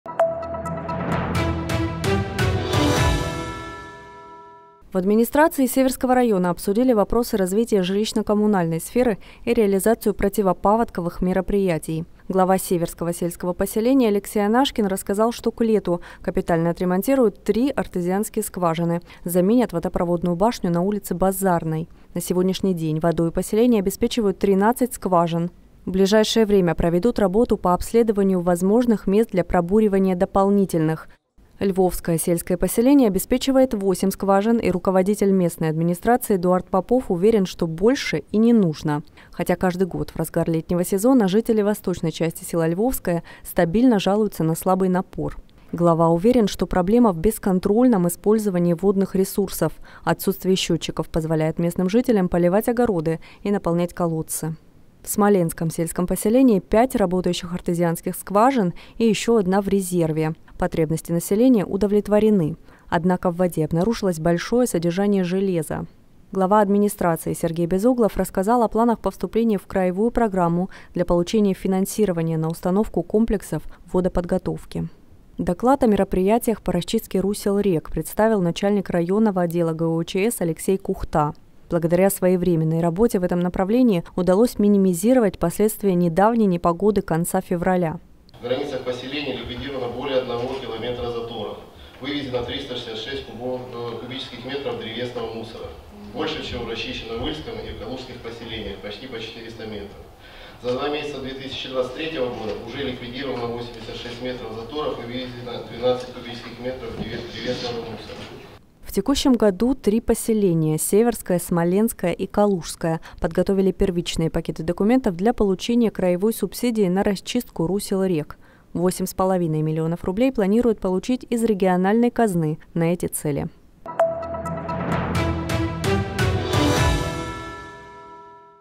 В администрации Северского района обсудили вопросы развития сферы ЖКХ
Глава Северского сельского поселения Алексей Анашкин рассказал, что к лету капитально отремонтируют три артезианские скважины, заменят водонапорную башню на улице Базарной. На сегодняшний день водой поселение обеспечивают 13 скважин. В ближайшее время проведут работу по обследованию возможных мест для пробуривания дополнительных, способных удовлетворить растущую потребность населения в воде.